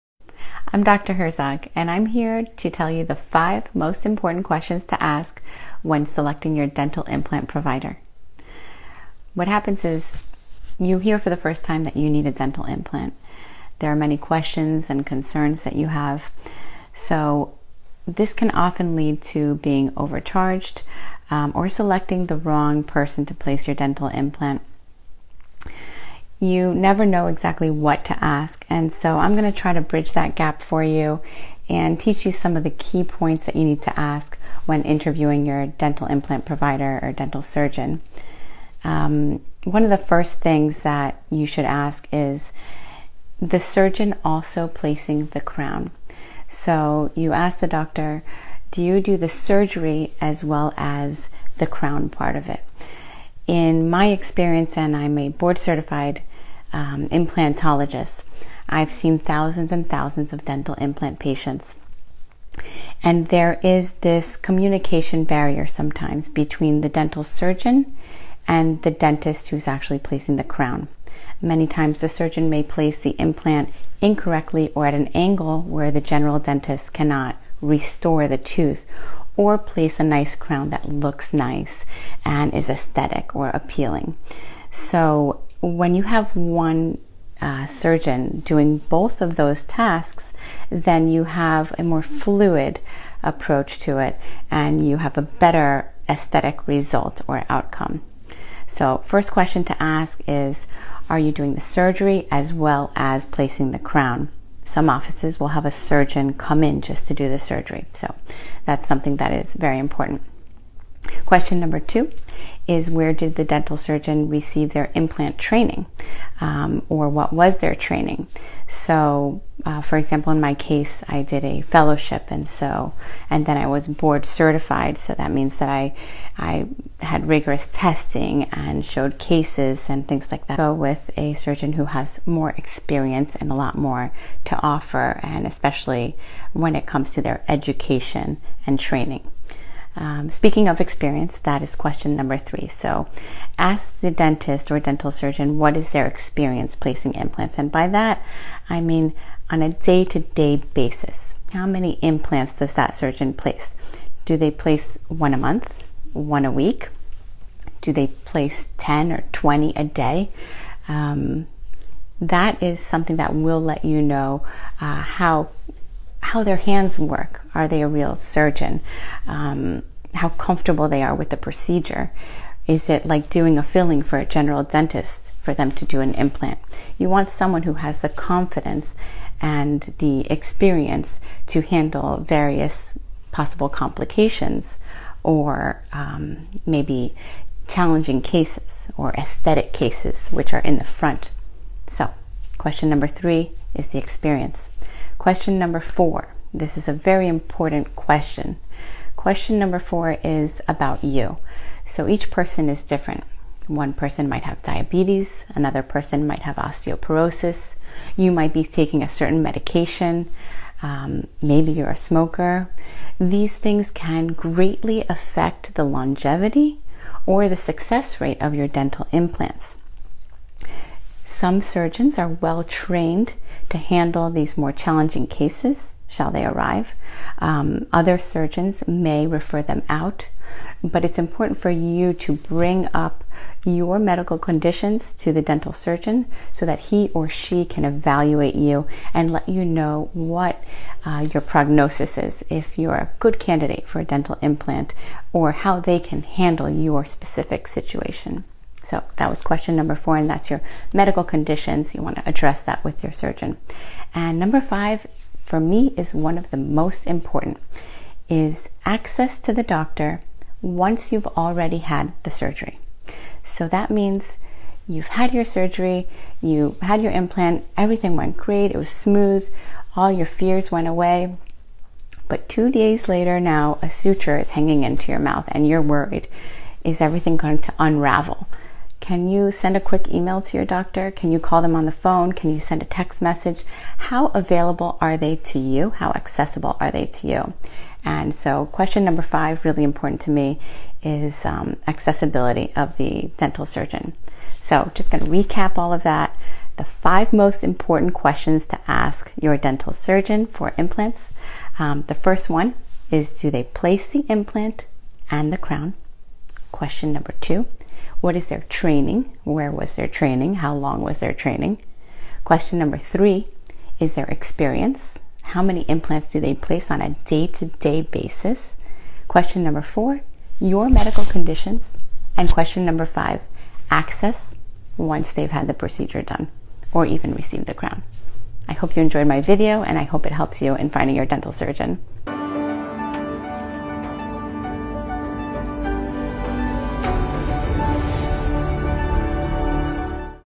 Top 5 Questions You Should Ask Your Implant Provider Before Surgery
Choosing the right implant provider can be the difference between a beautiful new smile or a costly regret. So, how do you choose the right dentist?

In this video, we share with you the top 5 questions you should ask your implant provider before committing to surgery. Stay informed, vet your dentist!